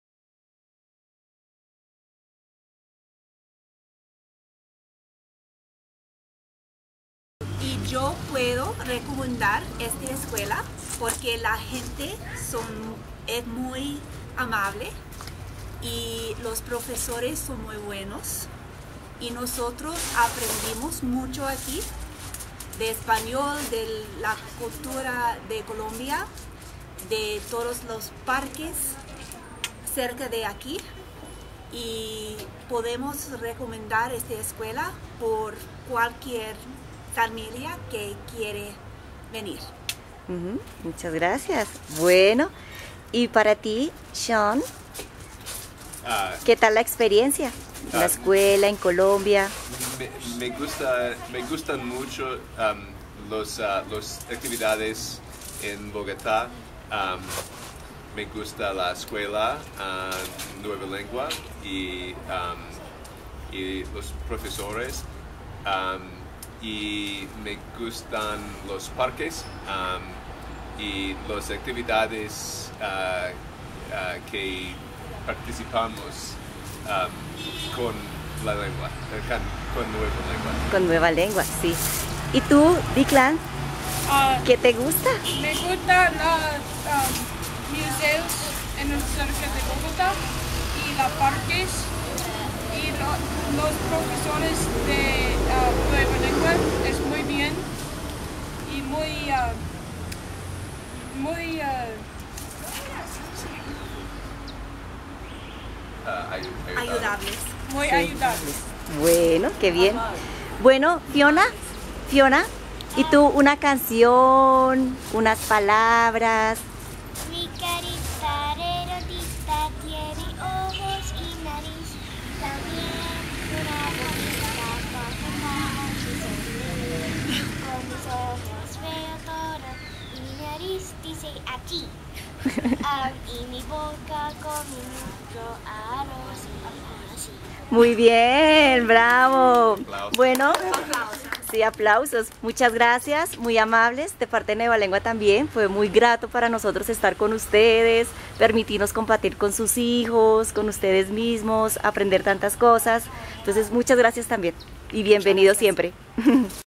Español en familia
La familia McLaughlin nos habla de lo que más le gustó de sus clases de español y de Bogotá. Siempre será un gran placer para nosotros recibir su visita.